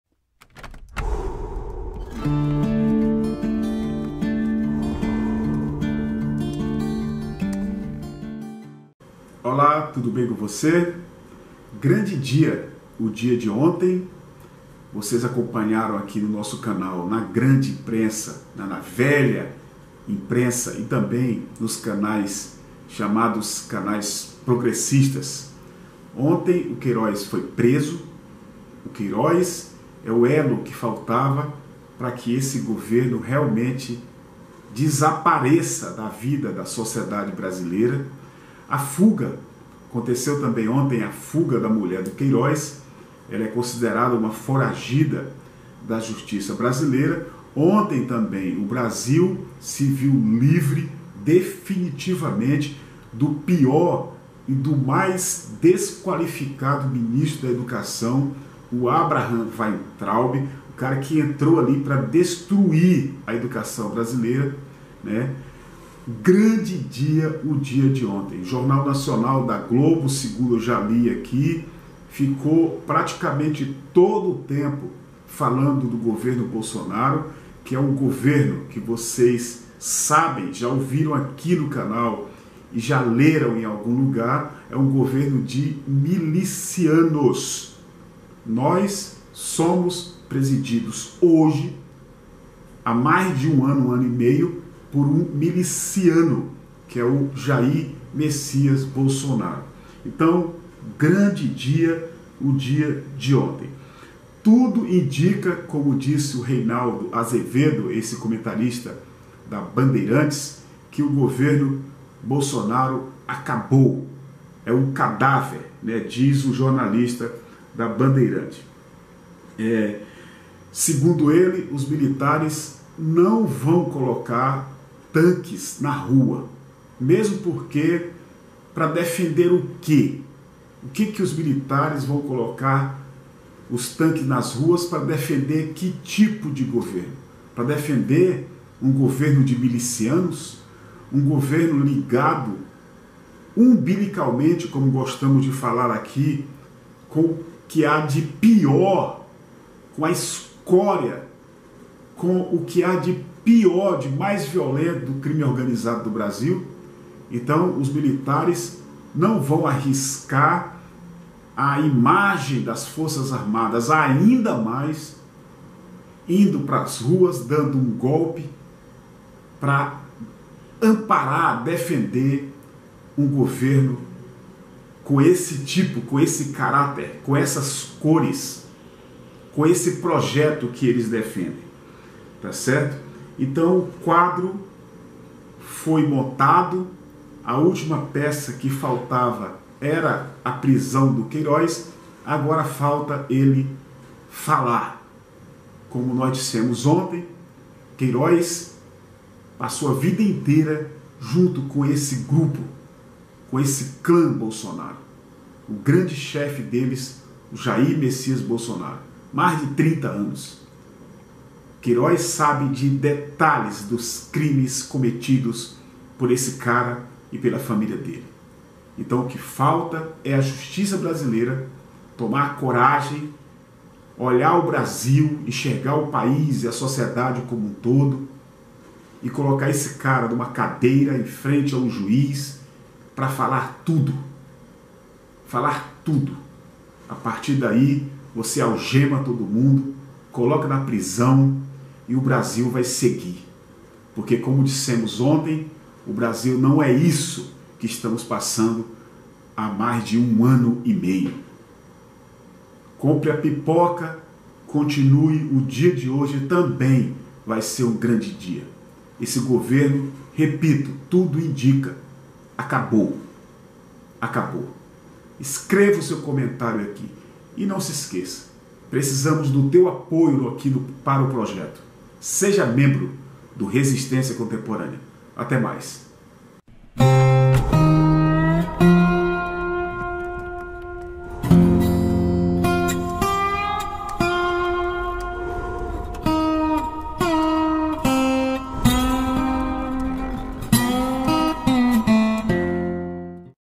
Acabou Bolsonaro! Queiróz Vai Expor o Esgoto!
A prisão de Fabricio Queiroz é consequência direta da tomada de posição do Supremo Tribunal Federal (STF), através das decisões do Ministro Alexandre de Moraes. Agora os demais órgãos de investigação começam a desovar seus inquéritos.
GGN

APOIE O CANAL AQUI:
CONTA PARA CONTRIBUIÇÃO:
Agência: 3314
Operação: 013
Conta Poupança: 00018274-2